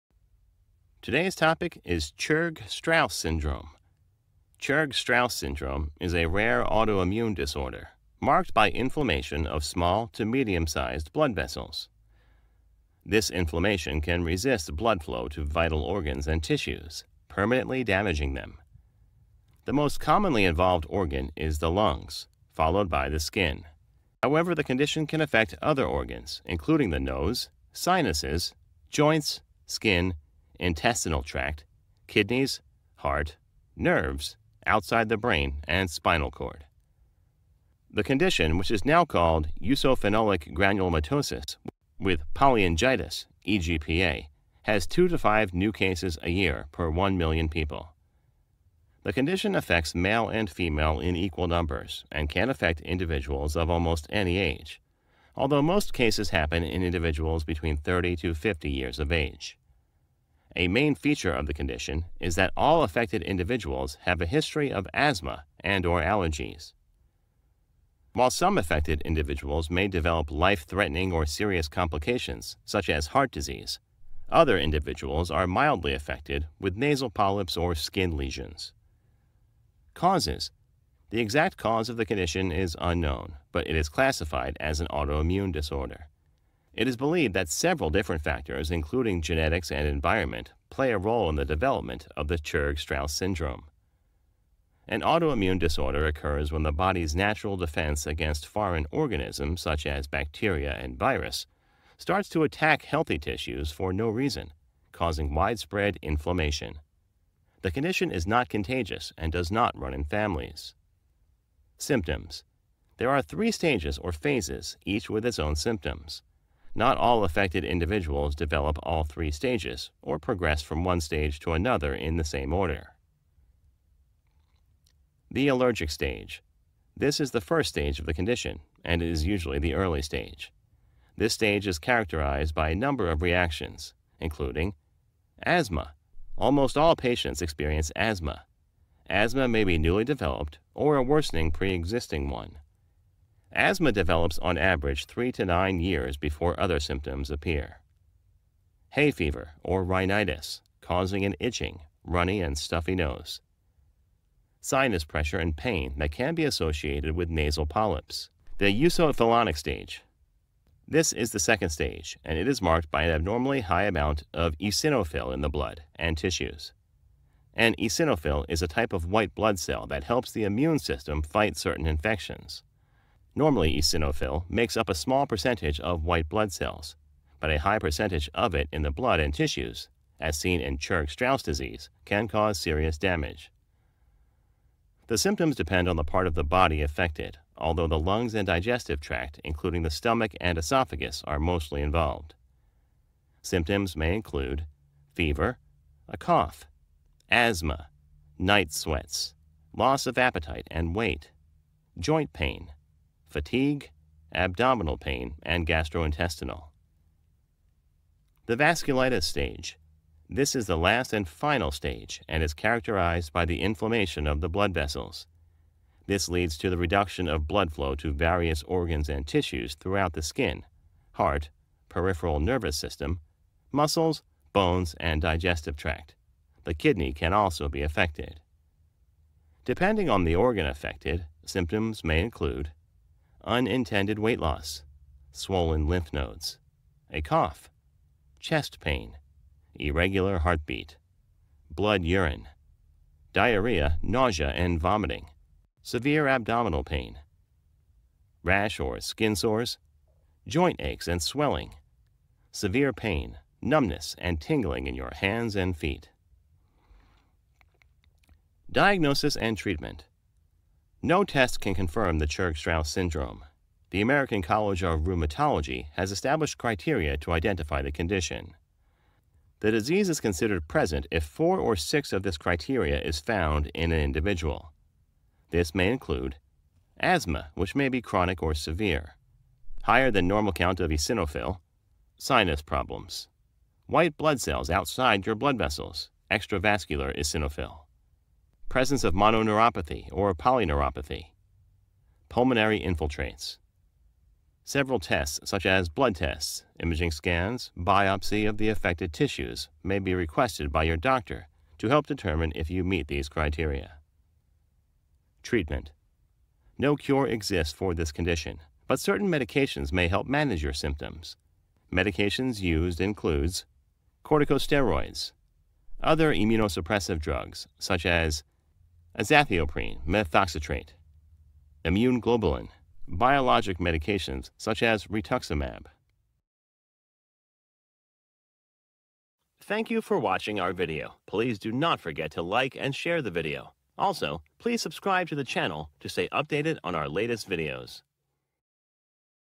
CHURG STRAUSS SYNDROME, Causes, Signs and Symptoms, Diagnosis and Treatment.
Chapters

0:00 Introduction
1:18 Causes of Churg-Strauss syndrome
1:51 Symptoms of Churg-Strauss syndrome
4:19 Diagnosis of Churg-Strauss syndrome
5:06 Treatment ofChurg-Strauss syndrome

Churg-Strauss syndrome is a disorder marked by blood vessel inflammation. This inflammation can restrict blood flow to vital organs and tissues, sometimes permanently damaging them. This condition is also known as eosinophilic granulomatosis (gran-u-low-muh-TOE-sis) with polyangiitis (pol-e-an-jee-I-tis).

Asthma is the most common sign of Churg-Strauss syndrome. The disorder can also cause a variety of other problems, such as hay fever, rash, gastrointestinal bleeding, and pain and numbness in your hands and feet.

Churg-Strauss syndrome is rare and has no cure. But your doctor can usually help you control symptoms with steroids and other powerful immunosuppressant drugs.